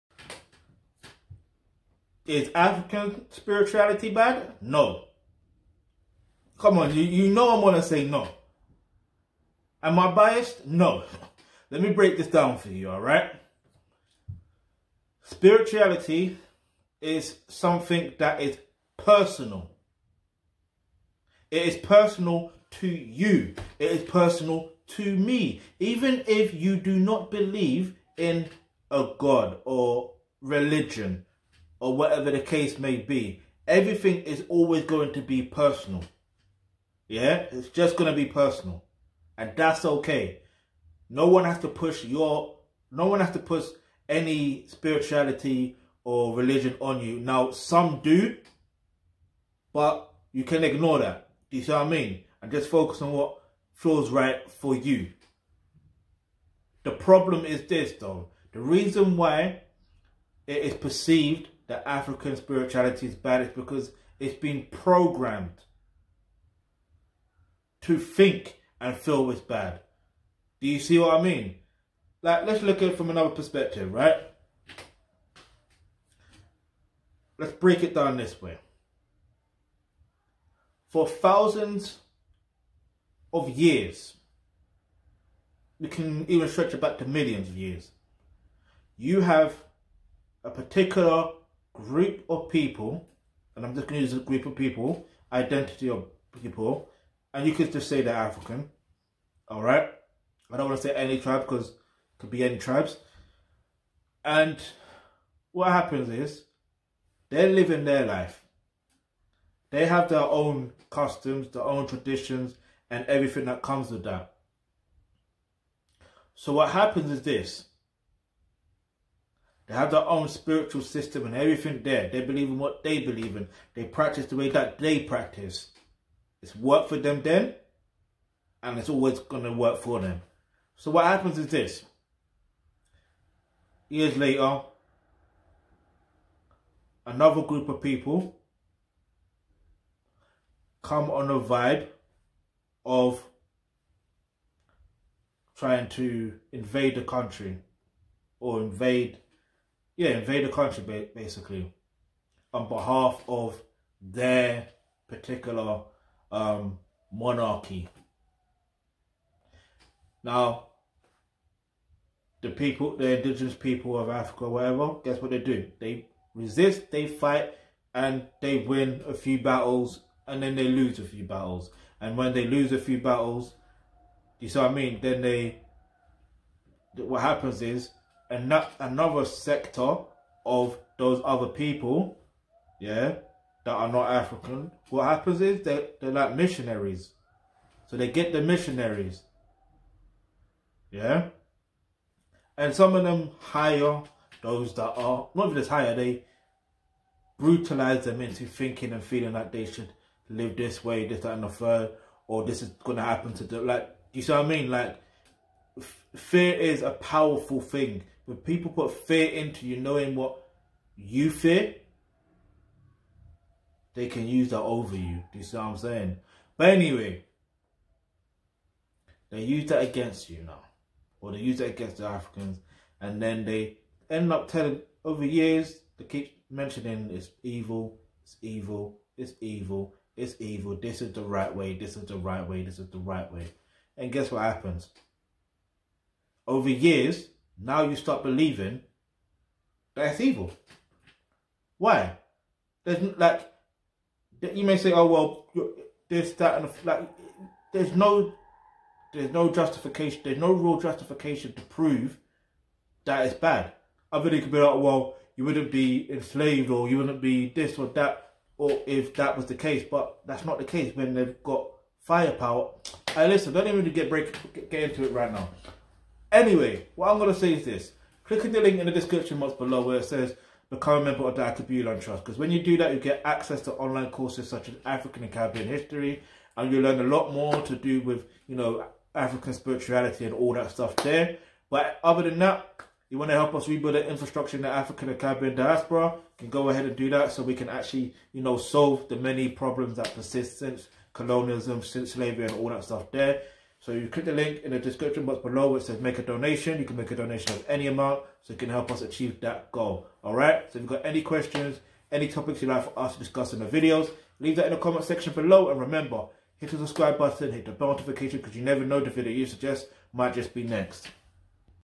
Is African Spirituality bad?
On the RoadTo1Billion Subscribers...in this video RaphaelOnochie aka Versatile Onochie-Obayuwana explains whether African Spirituality is bad or not...